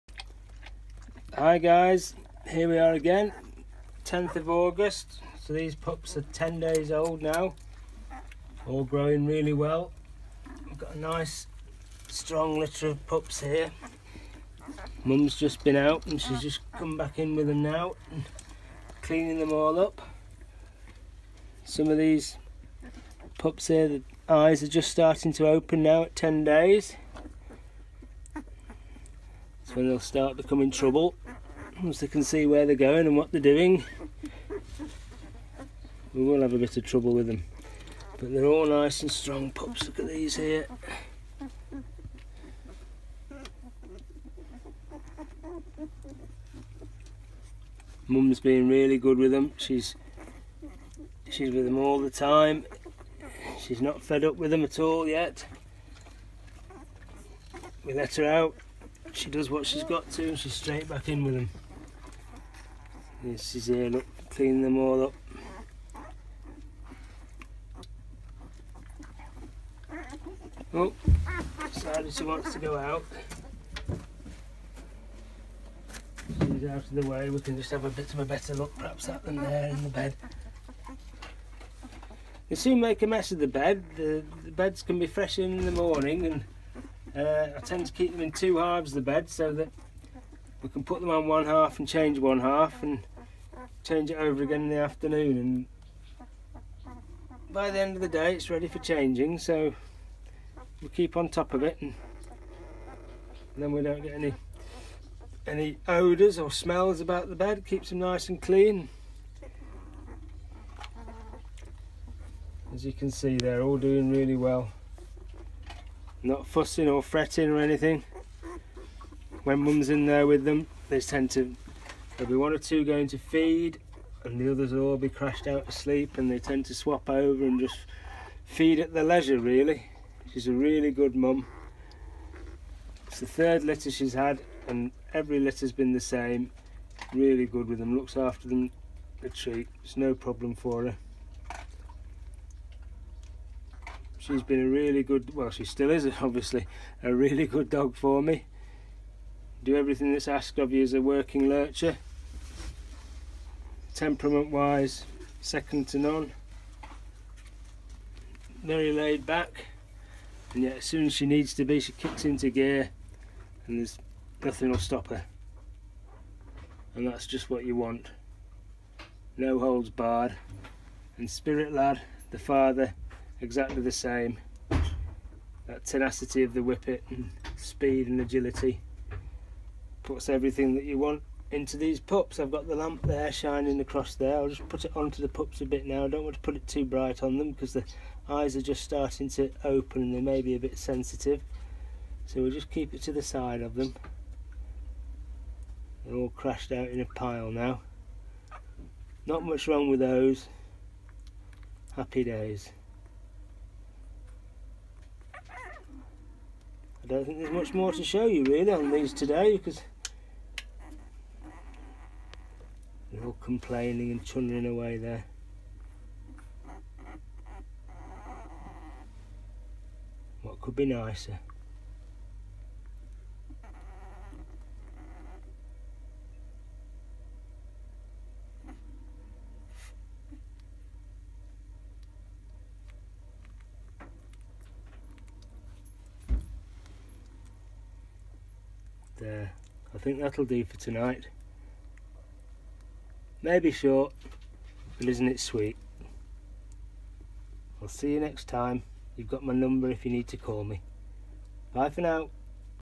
Dave's Deerhound Lurcher Pups - 10 days old
Deerhound/Greyhound x Whippet Lurcher Pups for sale
Pups bred from the best working dog lines